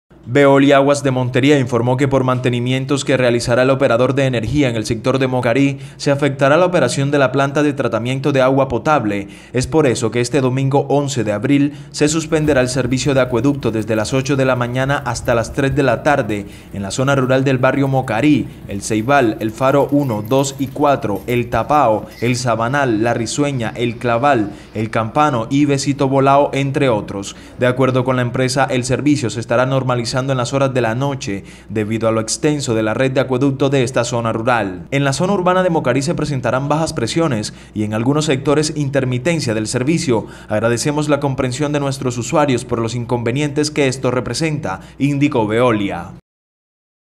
Este domingo se suspenderá servicio de acueducto en zona rural de Mocarí
Veolia Aguas de Montería informó que por mantenimientos que realizará el operador de energía en el sector de Mocarí se afectará la operación de la Planta de Tratamiento de Agua Potable.

 ¡Siempre Informando! 📢